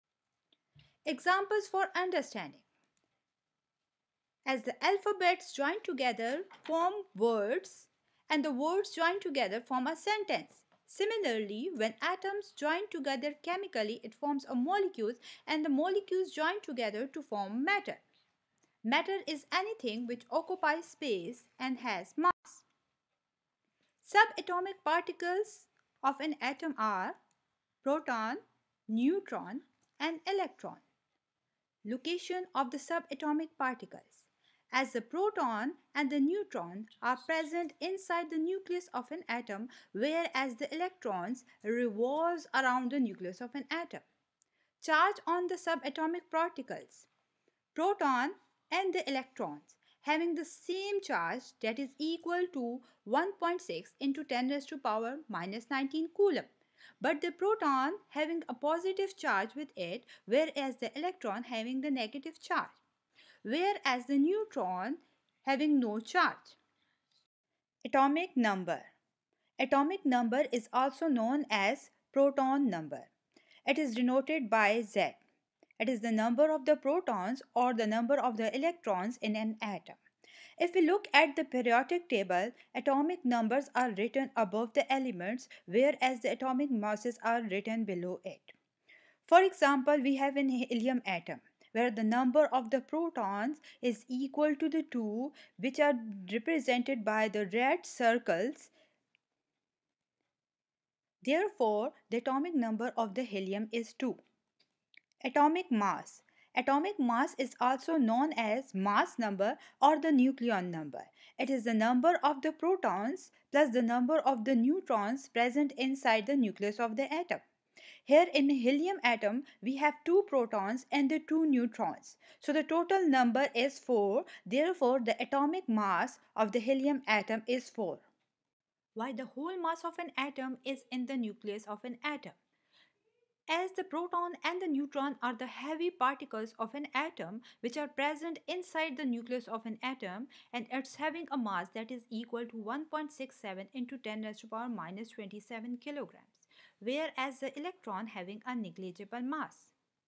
How to find the | Electrons ,Protons and Neutrons of an Element | Mass Number and Atomic Number
This video will help you in understanding the basic structure of the atom & molecules and fundamental subatomic particles.Also learn that what is Atomic number or Proton number, atomic mass or mass number or nucleon number in an easy animated way,Difference between atomic number and atomic mass with examples.How to find the number of protons, neutrons, and electrons in an element or atom. Explain the charge on electron, proton, and neutron. Also explain the mass of electron, proton, and neutron.
explain the structure of an atom diagrammatically.
examples of atomic number and mass number of helium.